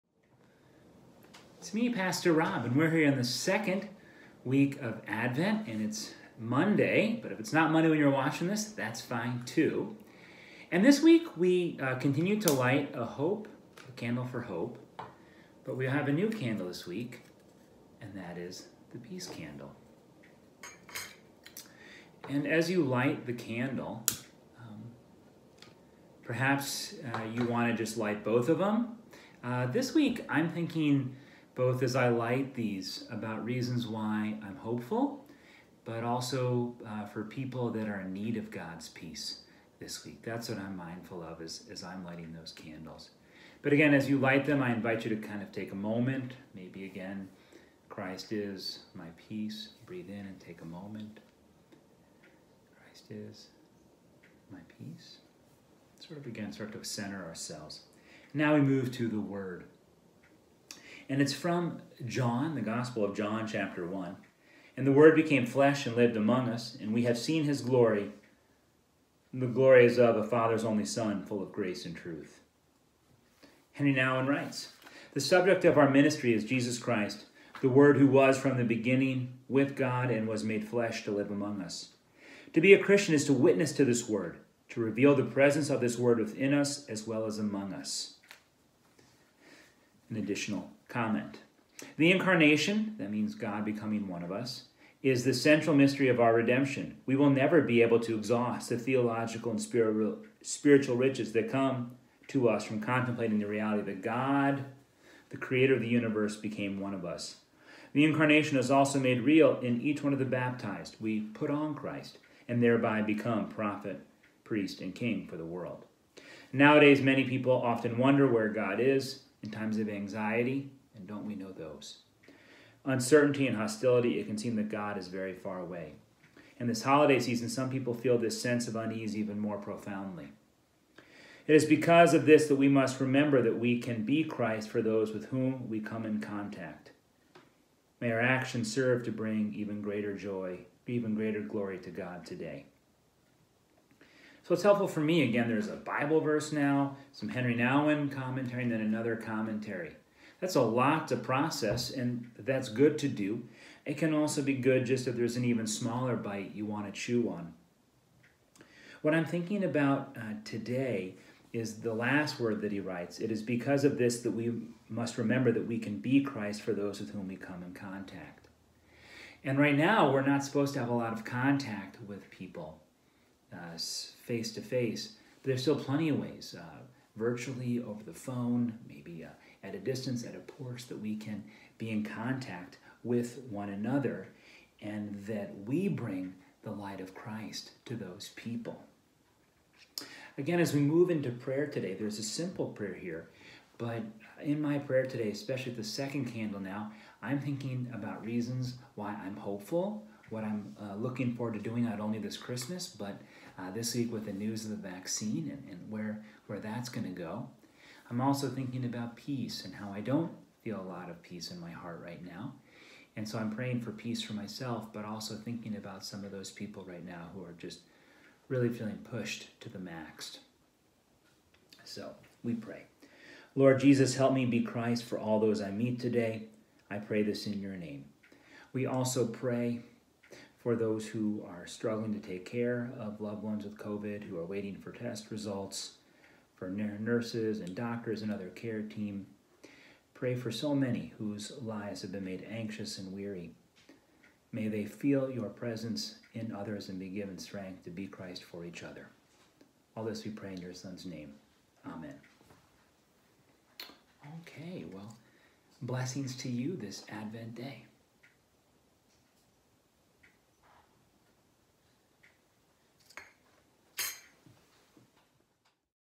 Advent 2 Monday Peace
Pastor Rob reflects on how we can be Christ for those with whom we have contact in a time when contact is discouraged.